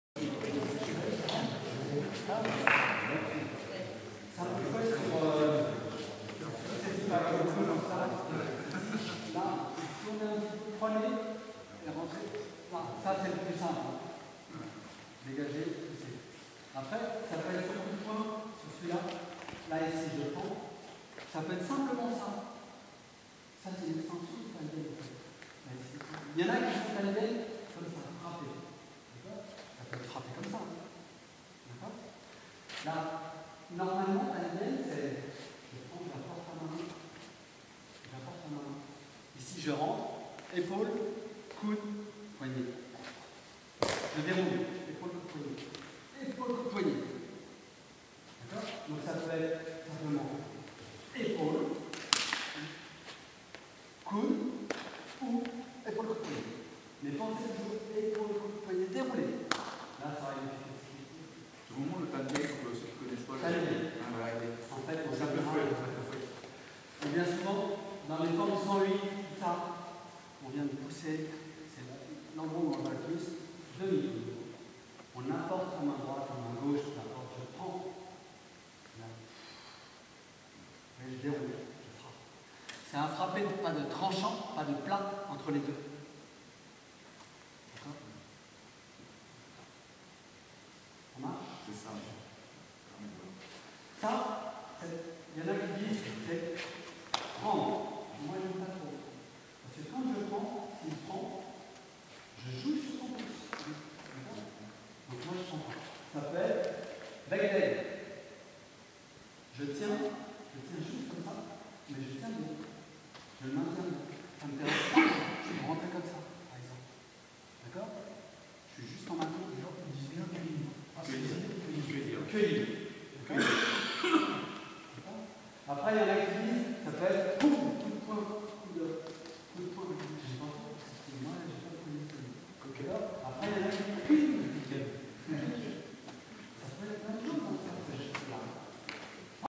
Gong Fang Belle Isle en Terre 2016 Simple fouet
Stage Gong Fang en Bretagne organisé par l'EPAM et le BKDO.

Applications martiales de mouvements du Tai Chi dirigé par Bertrand GAGNEUX